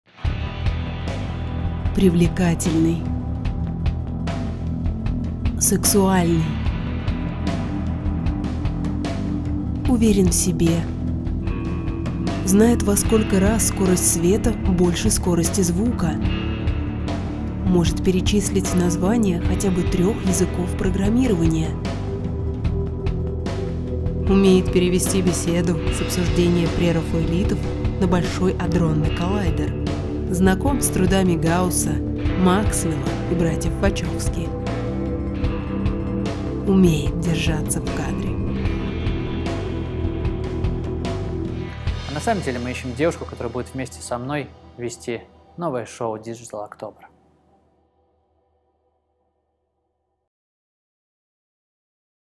Кастинг или Как надо подходить к поиску телеведущей
Ищем сногсшибательную соведущую для сногсшибательного вечернего развлекательного шоу (о технологиях и не только).

P.S. А работать вам предстоит с ведущим "Дождя" и сценаристом "Прожекторперисхилтон" Александром Филипенко (это он на видео).